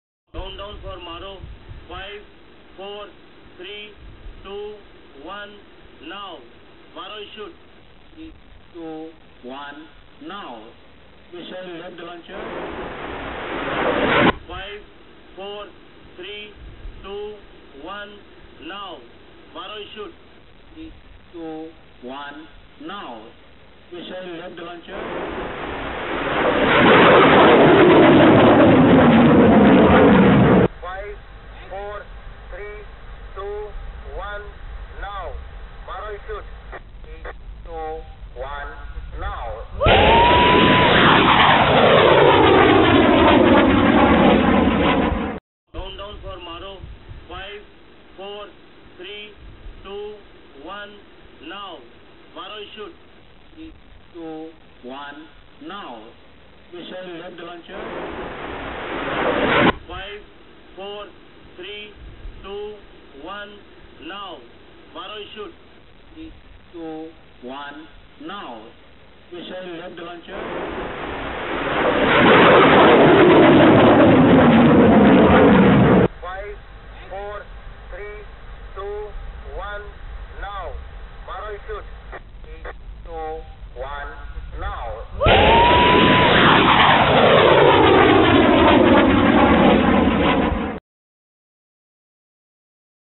DRDO successfully test-fires Quick Reaction Surface-to-Air Missile
India successfully test-fired Quick Reaction Surface-to-Air Missile. It was fired from a test range off the Odisha coast. QRSAM system has been developed by DRDO. It is expected to be ready for induction by 2021.